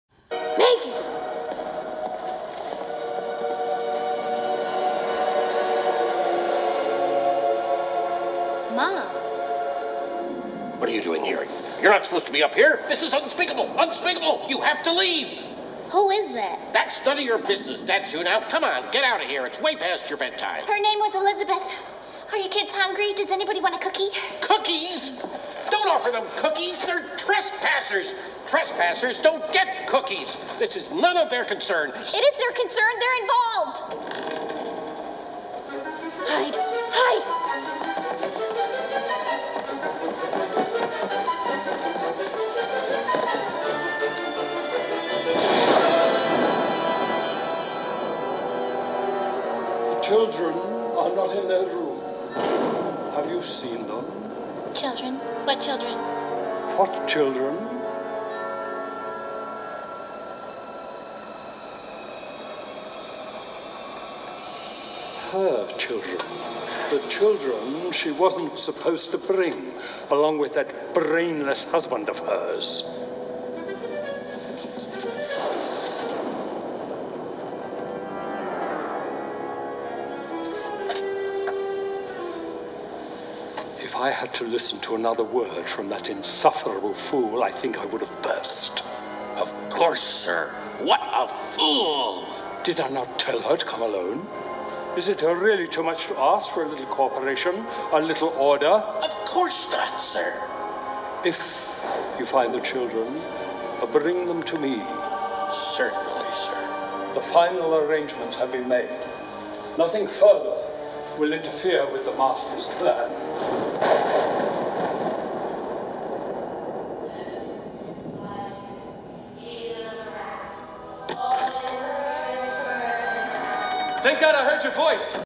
Steven Universe-The Haunted Mansion-Part 13: Emma And Ezra
The next part of The Haunted Mansion!

Cast:
Jim-Peridot
Megan- Connie
Michael- Steven
Ramsley- Garnet
Emma- Pearl
Ezra- Amethyst